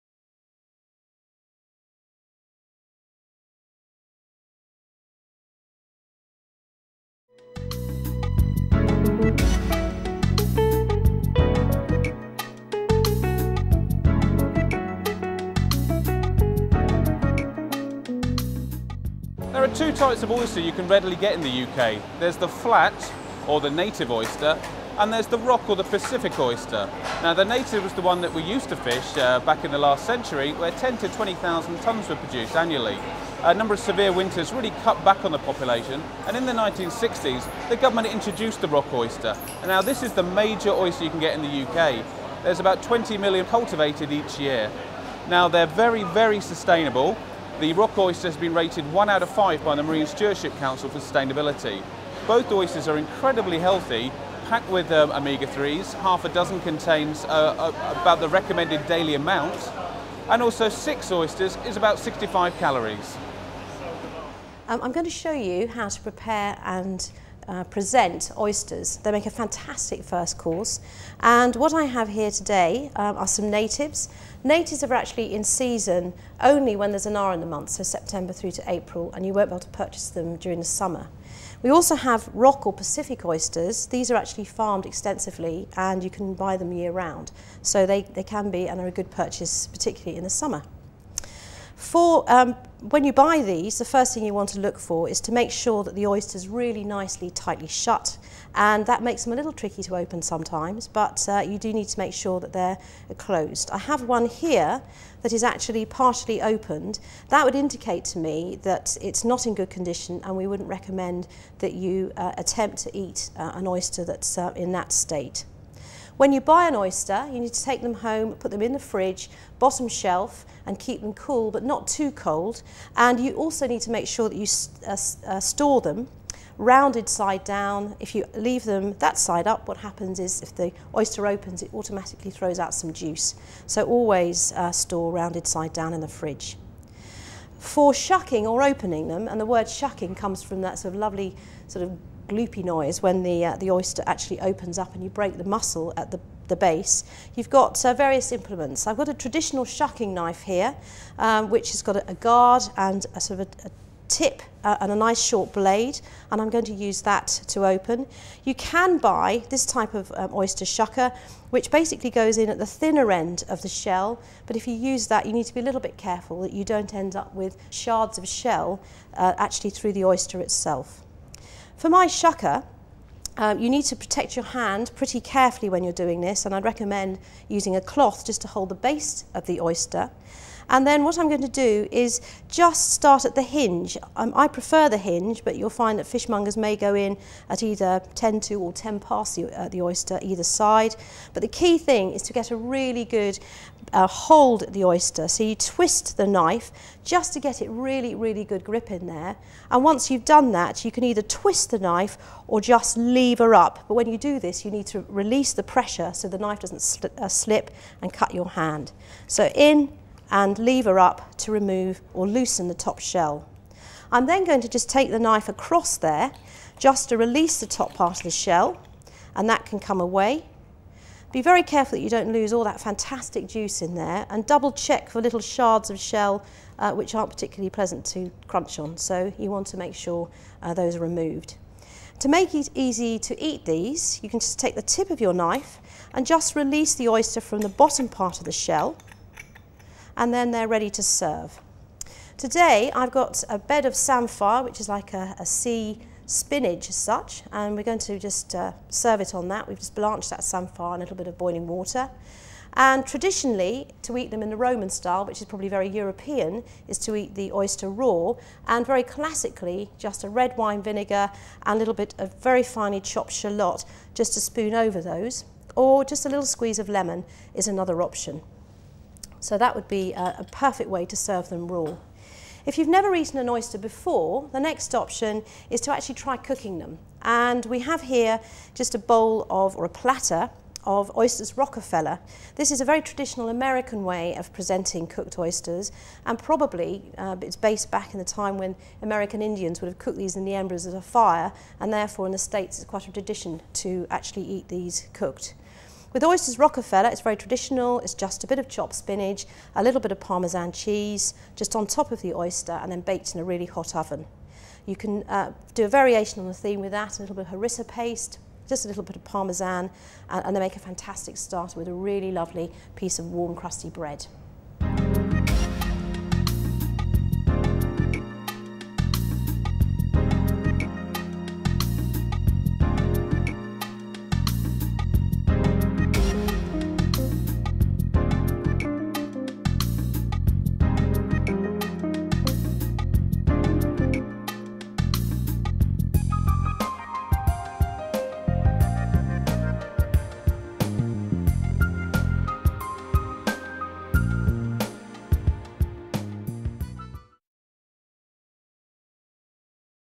How to Open Oysters
One video from a series of short films that aim to educate and raise awarness of edible shellfish within the UK